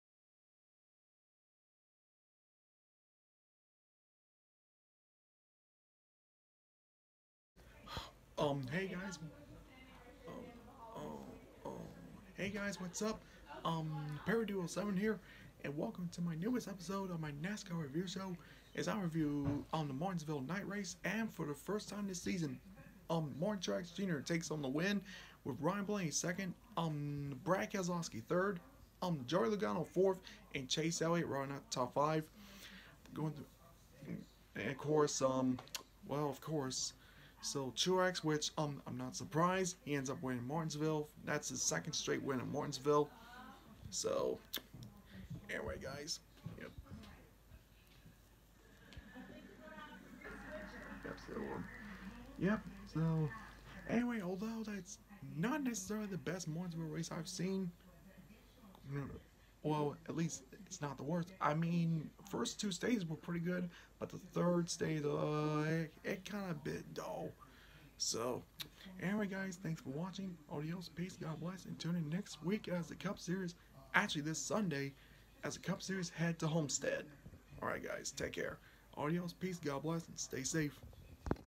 My NASCAR Review Show! Episode 253
My thoughts on Martinsville! Enjoy the video!